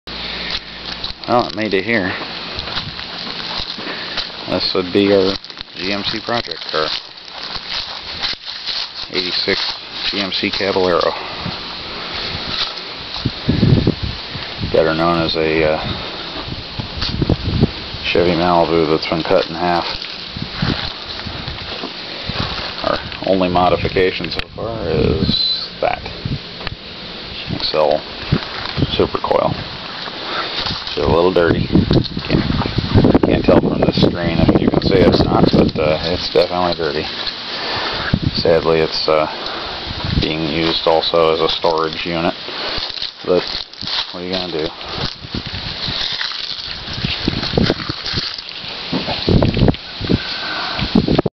Showing the Caballero
Nothing real special this go around, just showing a vid of our father/son project truck. It's a 1986 GMC Caballero, 305ci V8. Just a 'tad' dirty from sitting, but still a good vehicle. We just need to have more money so we can work on it more!